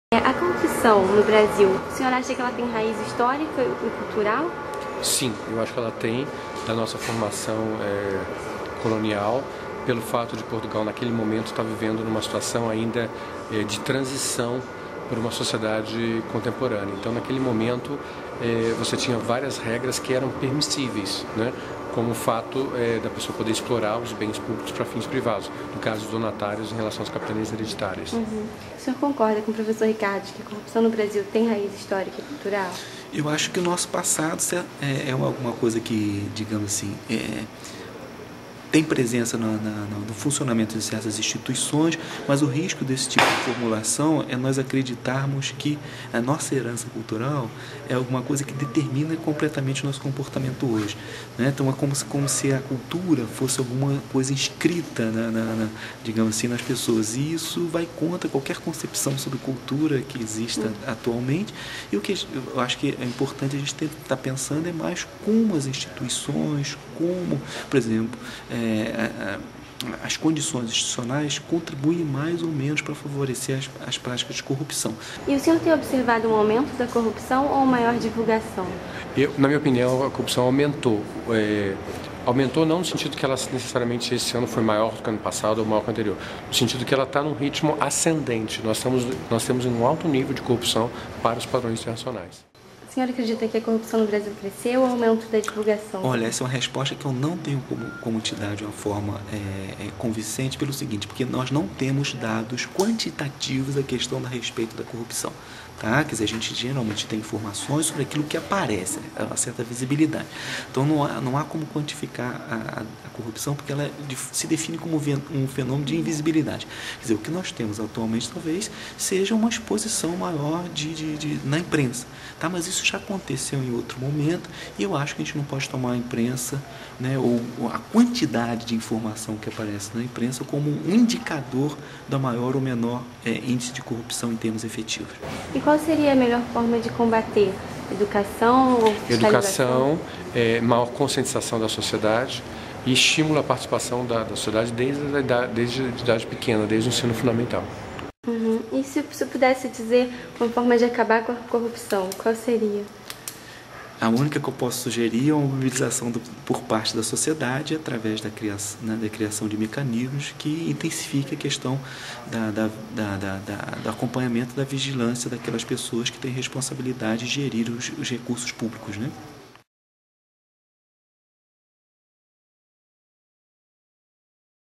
O custo da corrupção
Cientistas políticos divergem sobre as causas e as soluções desse problema que afeta o cotidiano do país. Enquanto um enxerga causas históricas e culturais para a questão, outro acredita que o próprio sistema político atual a favorece.